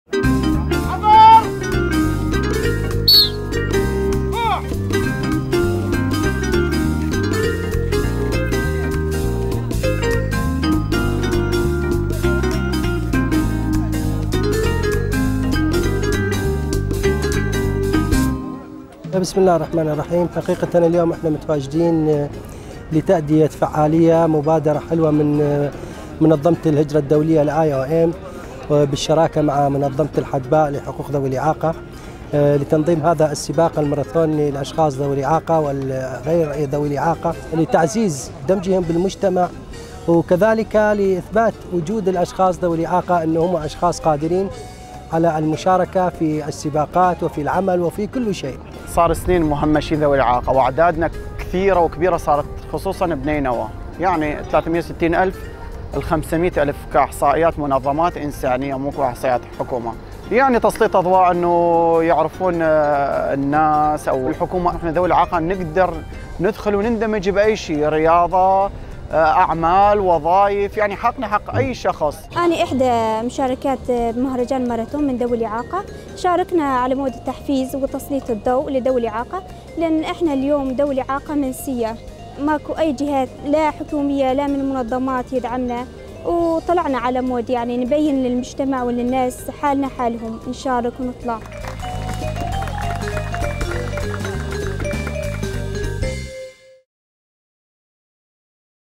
فعالية رياضية لذوي الاعاقة في نينوى
احتضنت الموصل فعالية رياضية لذوي الاعاقة بتنظيم من منظمة الحدباء ومنظمة الهجرة الدولية لتعزيز دور ذوي الاعاقة في المجتمع.